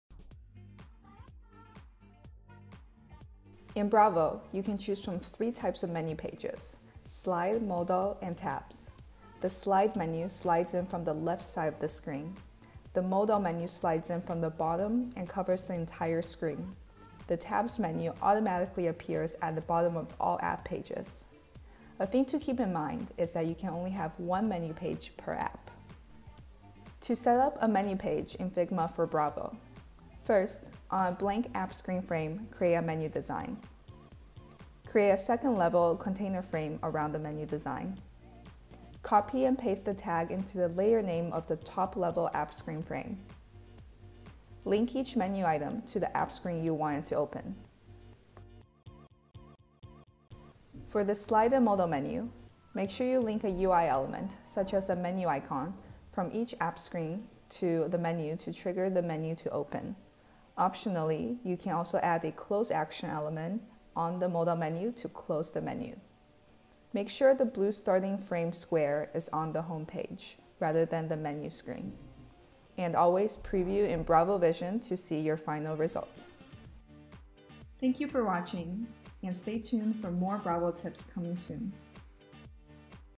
Bravo Tips #2: Menu Types
In this second Bravo Tip episode we introduce you to the types of menu navigation available.

Start Bravorizing today!